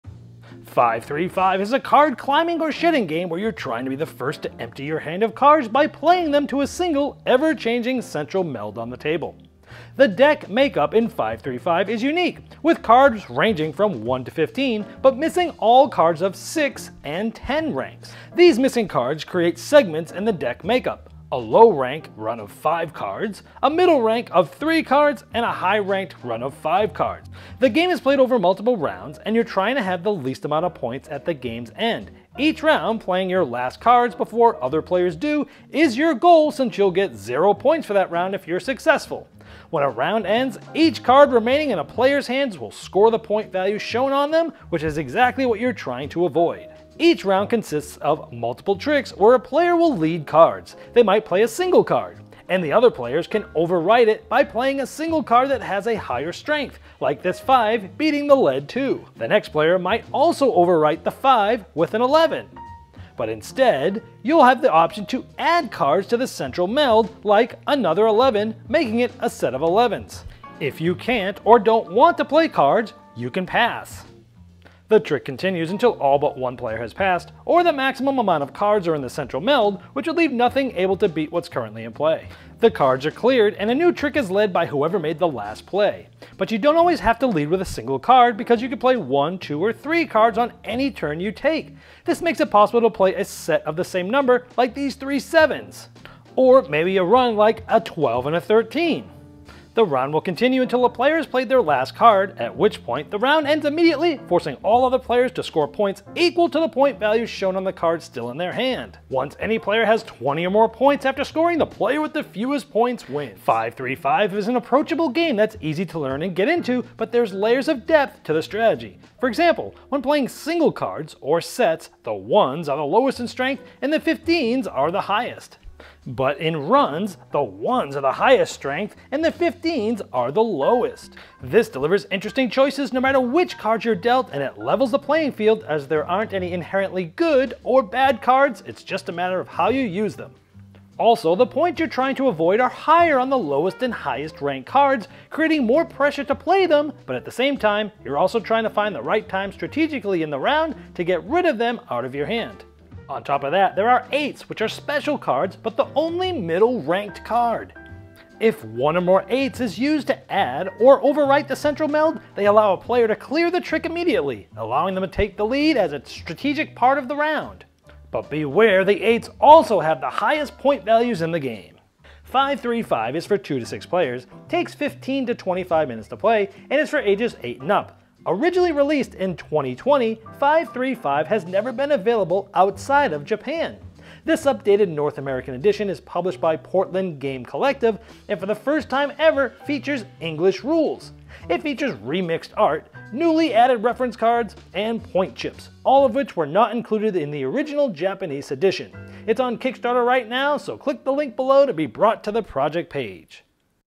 Five Three Five (English Edition of 535 from Japan) Quick Overview
See how the card climbing / shedding game Five Three Five works, what makes it different from other games in the genre, and what makes it fun!